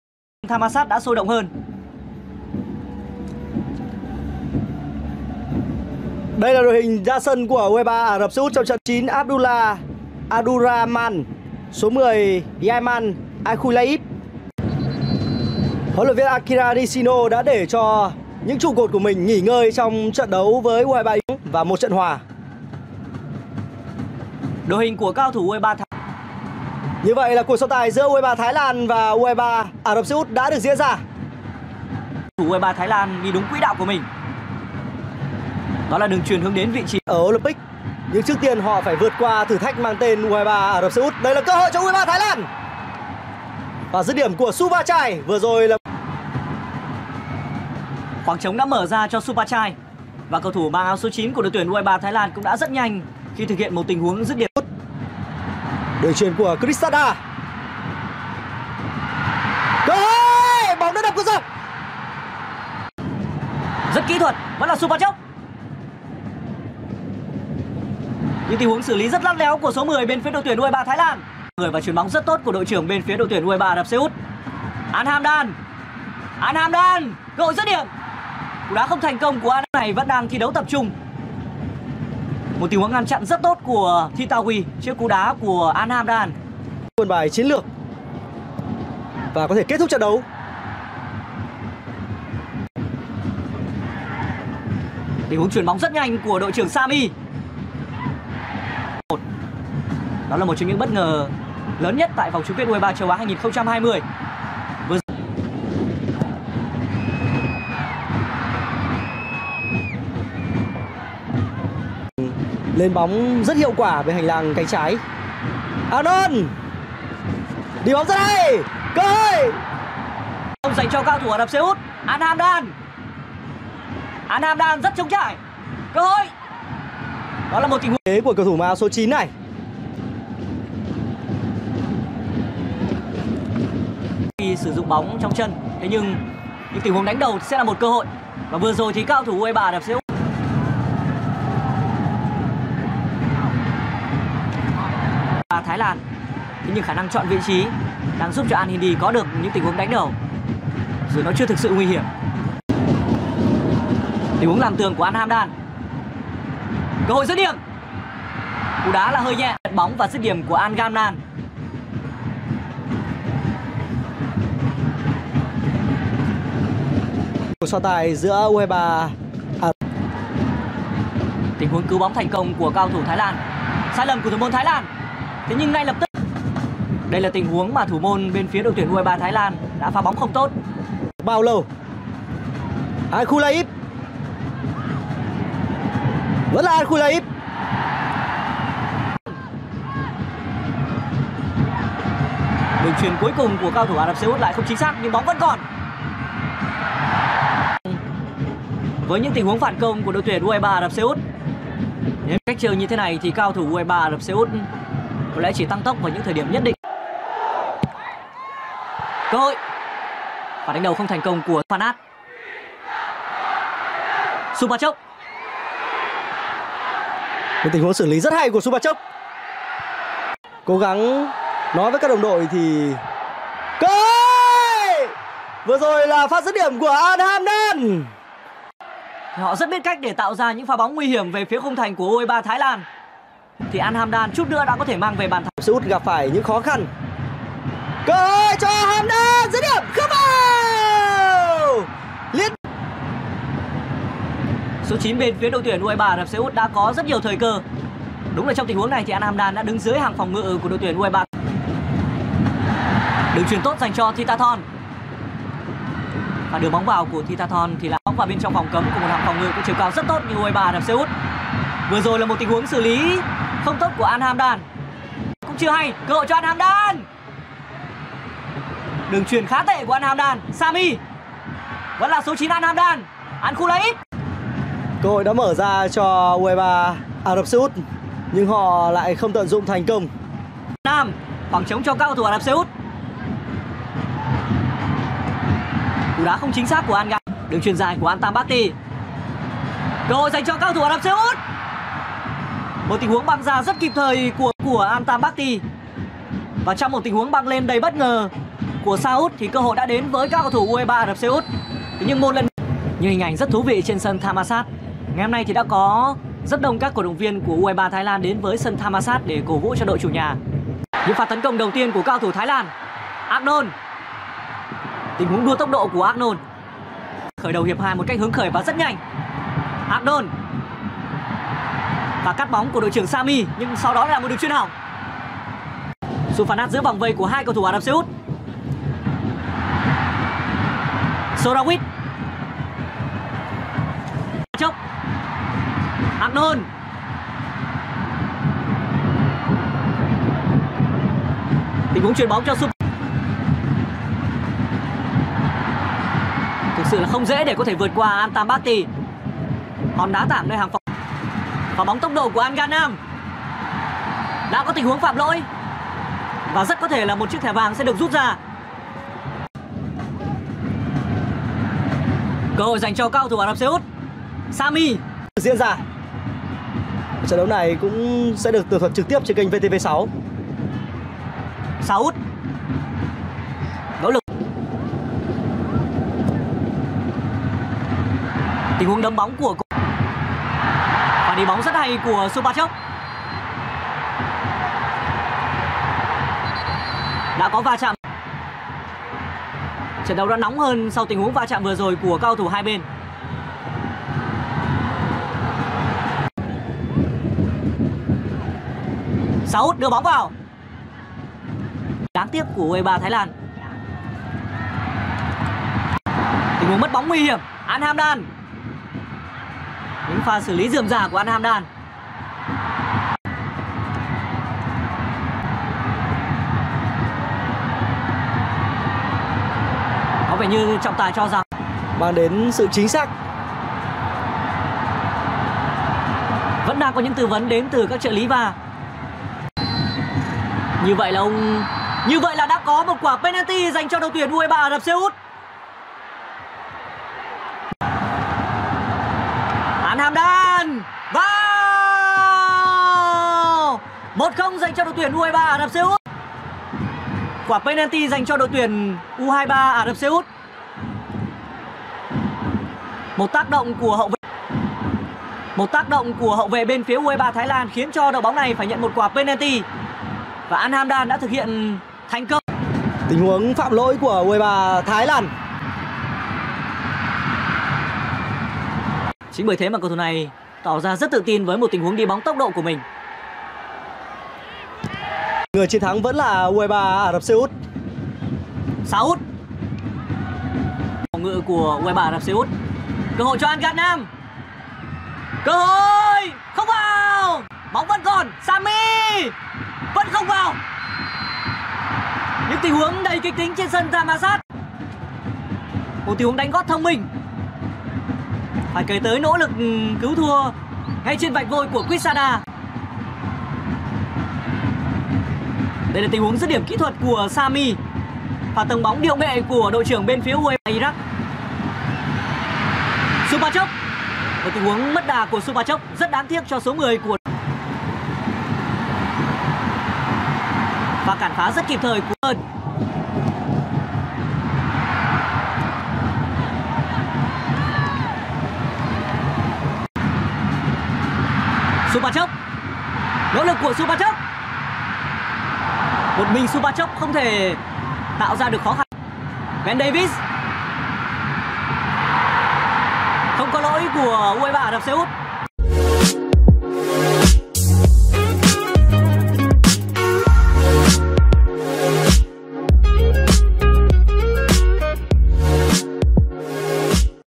U23 Saudi Arabia vs U23 Thailand Highlights: Lỗi tại VAR
Bị đánh giá thấp hơn, nhưng U23 Thái Lan mới là đội thi đấu lấn lướt U23 Saudi Arabia. Nếu Supachai Jaided tận dụng thành công sai lầm của thủ môn đối phương (phút thứ 2) hay cú dứt điểm của Suphanat (phút thứ 6) không đưa bóng đi trúng cột dọc, "Voi chiến" đã có thể ăn mừng sớm.
Sau những phút đầu lép vế, U23 Saudi Arabia dần lấy lại thế trận và dồn ép U23 Thái Lan ngược về phần sân nhà. Chỉ tiếc là trong một hiệp đấu vô duyên đến khó hiểu, các tiền đạo đại diện Tây Á liên tục bỏ lỡ cơ hội ăn bàn mười mươi.

Thế trận đôi công được duy trì sang tận hiệp 2. Dù vậy khi hàng công hai đội chưa thể tìm đường vào khung thành đối phương, bước ngoặt đã xảy ra. Phút 78, trọng tài chính Ahmed Al-Kaf nhờ tới sự hỗ trợ của VAR và thay đổi quyết định, cho U23 Saudi Arabia hưởng quả phạt đền thay vì đá phạt trực tiếp. Trên chấm 11m, Al Hamdan không bỏ lỡ cơ hội khai thông bế tắc.

Khoảng thời gian còn lại, không những không thể ghi bàn, thầy trò HLV Akira Nishino còn suýt nhận thêm 2 bàn nữa nếu cầu thủ U23 Saudi Arabia sắc bén hơn.

Dù sao đi nữa, kết quả 1-0 cũng giúp đại diện Tây Á tiến vào bán kết U23 châu Á, còn U23 Thái Lan chính thức khép lại giấc mơ dự Olympic Tokyo.